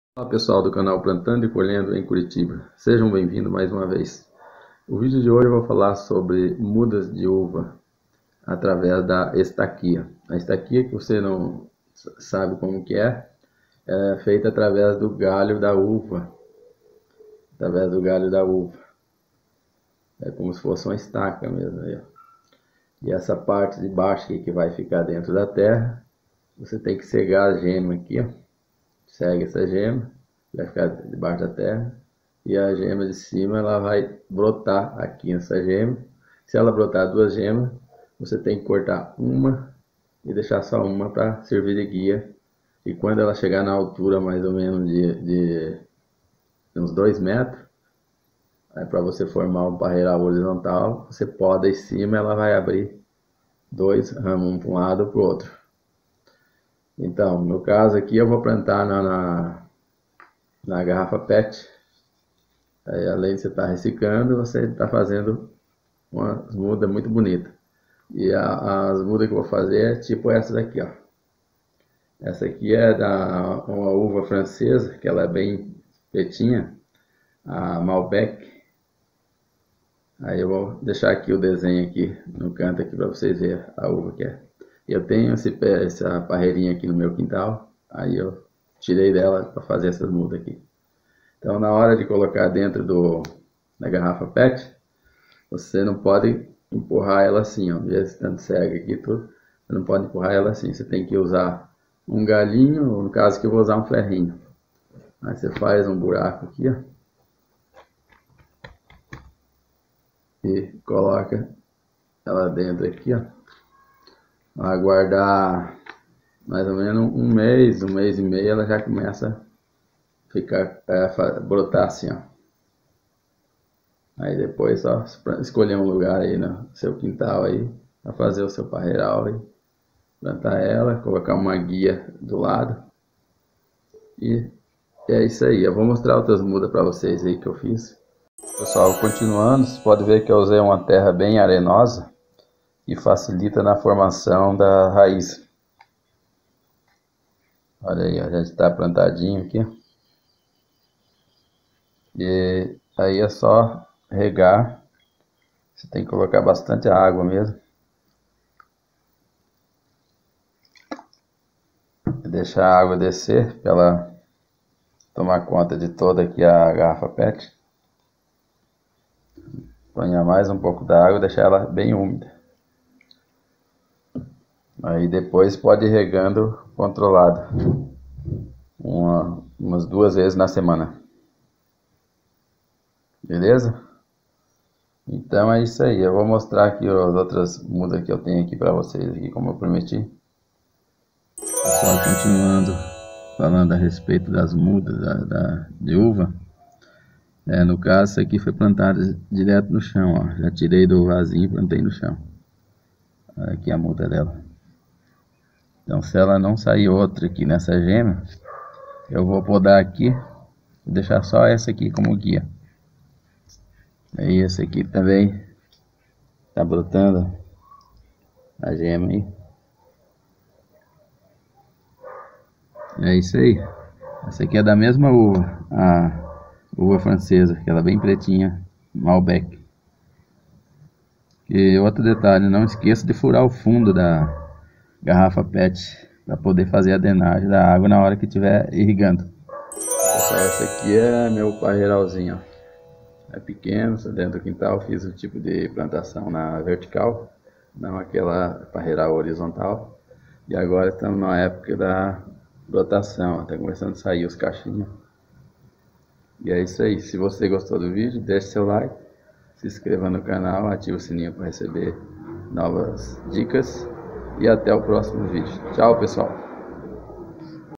como fazer mudas de uva por estaquia
tirei galho de uva para fazer mudas através
de estaquia e mostrei algumas mudas feitas...